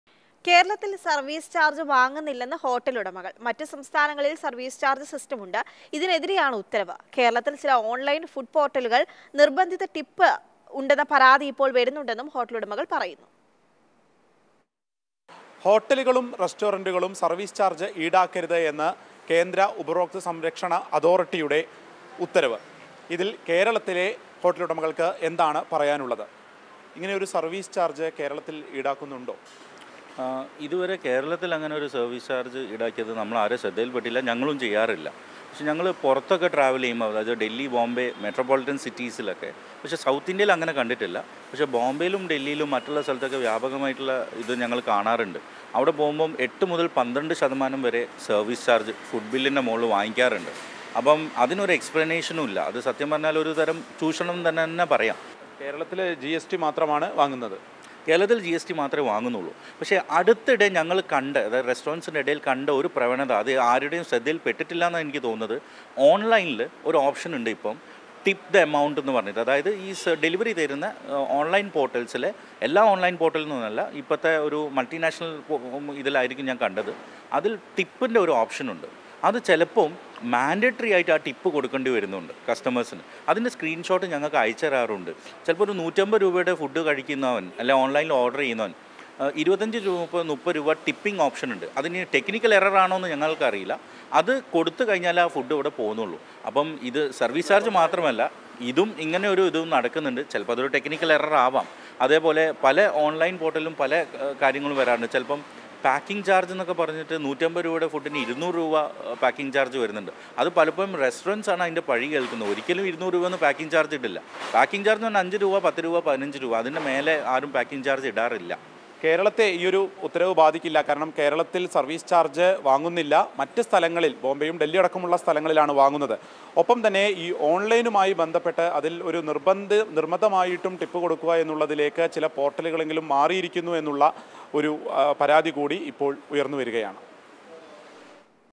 കേരളത്തിൽ സർവ്വീസ് ചാർജ്ജ് വാങ്ങുന്നില്ലെന്ന് ഹോട്ടലുടമകൾ | Mathrubhumi News
മറ്റു സംസ്ഥാനങ്ങളിൽ സർവ്വീസ് ചാർജ്ജ് സിസ്റ്റം ഉണ്ട്, ഇതിനെതിരെയാണ് ഉത്തരവ്. കേരളത്തിൽ ചില ഓൺലൈൻ ഫുഡ് പോർട്ടലുകളിൽ  നിർബന്ധിത ടിപ്പ് ഉണ്ടെന്ന പരാതി ഇപ്പോൾ വരുന്നുണ്ടെന്നും ഹോട്ടലുടമകൾ പറയുന്നു

Happy viewing!